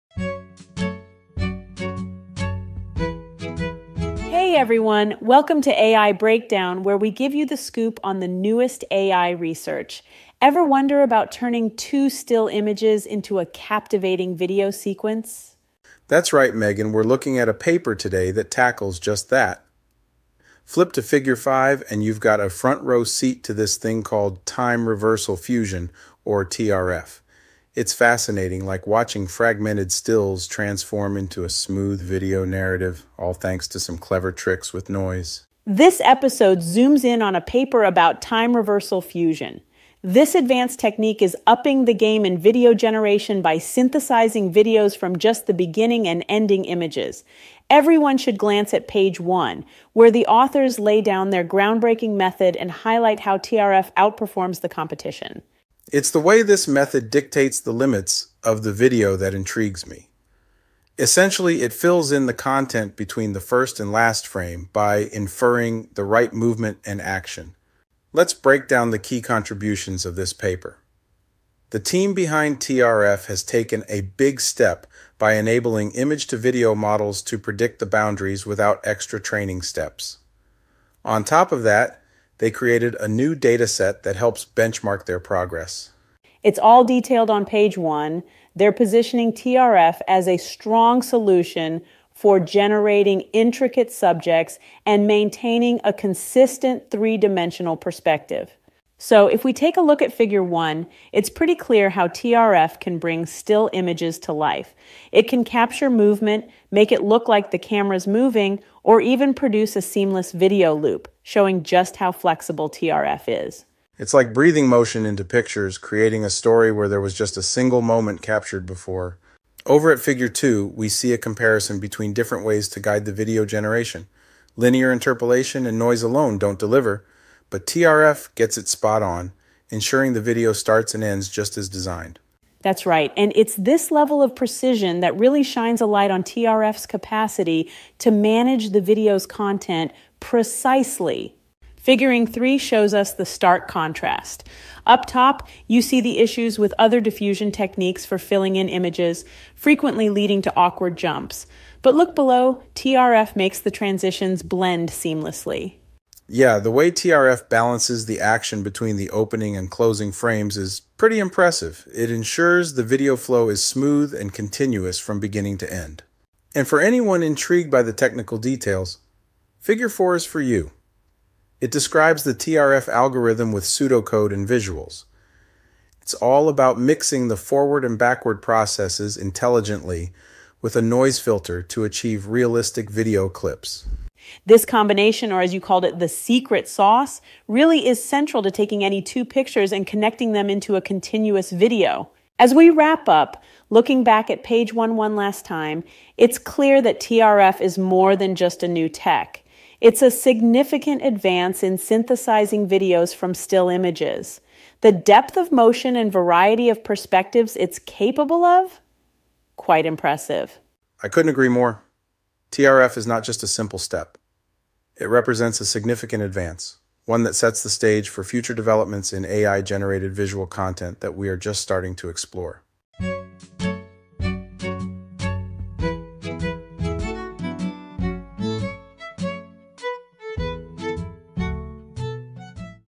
Explorative Inbetweening of Time and Space
In this episode, we discuss Explorative Inbetweening of Time and Space by:

- Haiwen Feng
- Zheng Ding
- Zhihao Xia
- Simon Niklaus
- Victoria Abrevaya
- Michael J. Black
- Xuaner Zhang. The paper presents a novel approach called bounded generation for creating intermediate video frames from a start and end frame, introducing Time Reversal Fusion (TRF) to fuse temporal paths for this purpose. TRF enables the use of existing image-to-video models for generating such videos without additional training. Empirical tests on a new dataset of 395 image pairs show that TRF outperforms existing methods in generating realistic and coherent videos within the bounds set by the given frames.